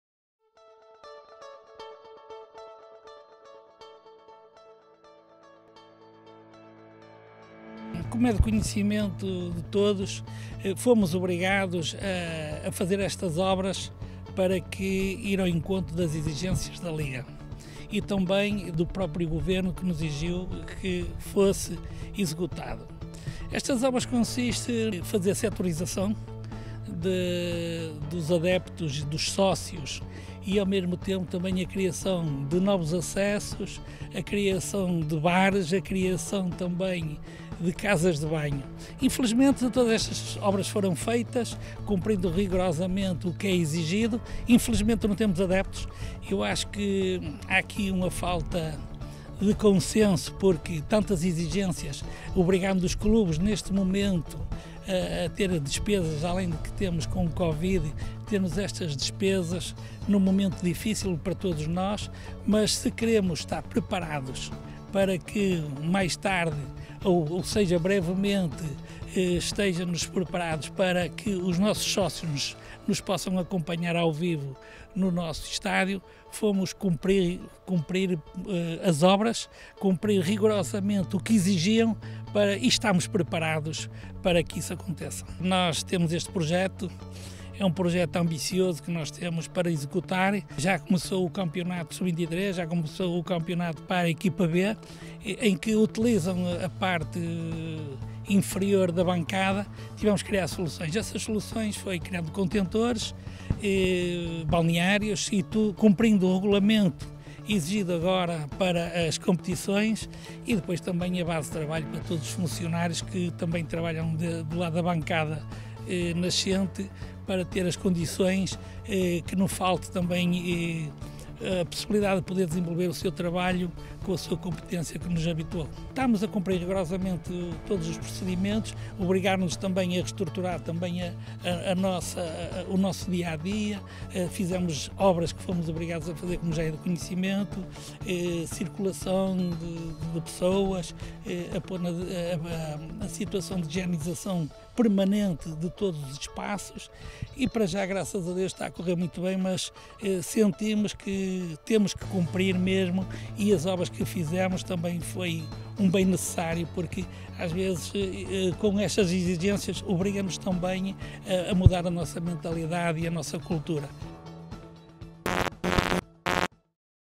Intervenções no Estádio do Rio Ave FC
Ao abrigo da legislação acerca da segregação de adeptos, das exigências impostas pela pandemia ou face às obras previstas para a demolição da bancada nascente, o Estádio do Rio Ave FC tem sido alvo de intervenções nas últimas semanas.

Desafios que têm sido ultrapassados com a tenacidade rioavista habitual e que o Presidente António da Silva Campos comentou, descrevendo as alterações realizadas.